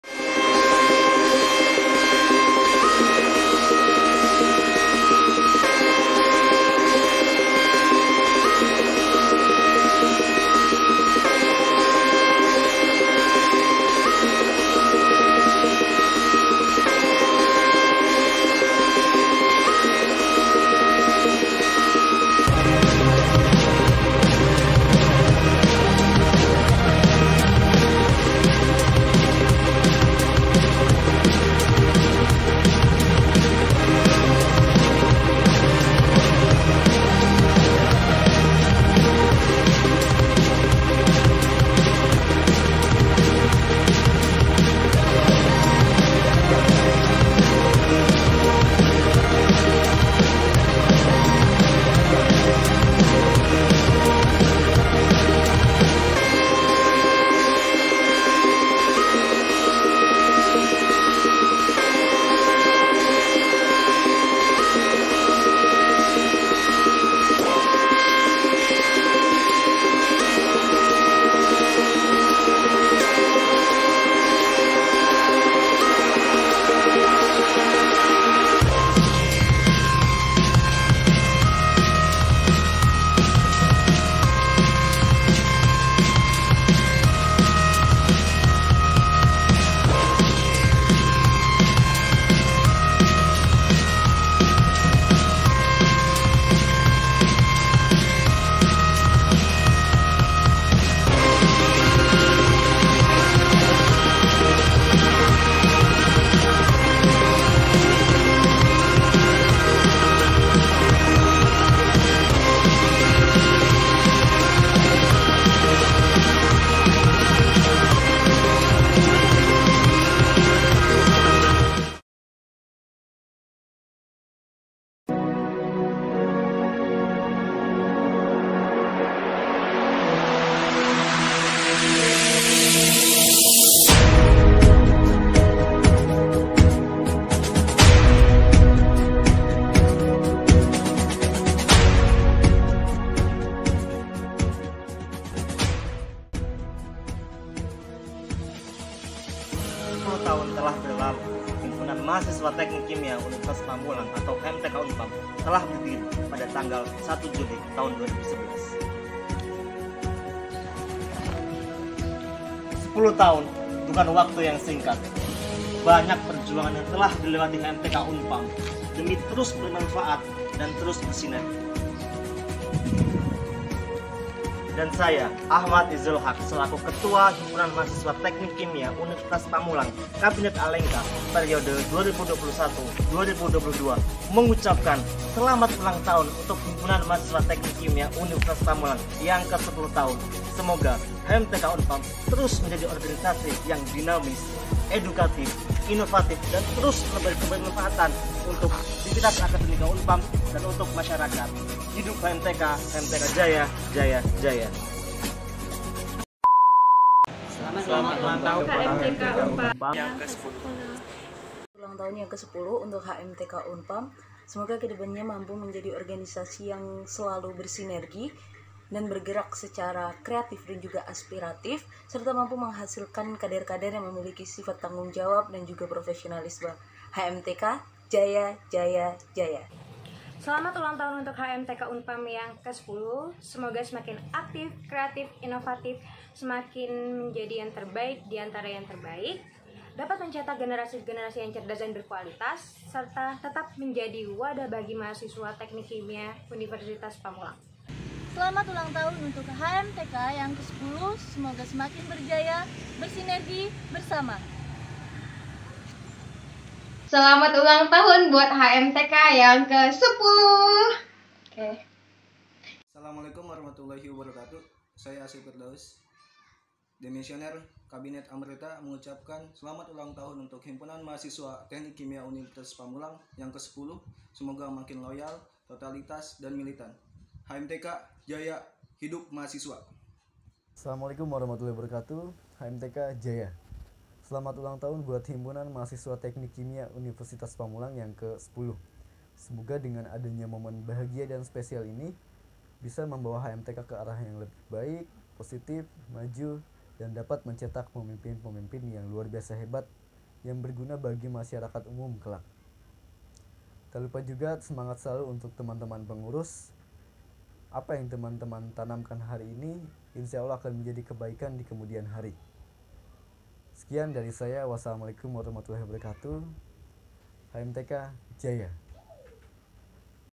Dies Natalis HMTK UNPAM 2021
Perayaan Dies Natalis HMTK UNPAM 2021 yang ke 10 tahun. HMTK UNPAM jaya jaya jaya!!!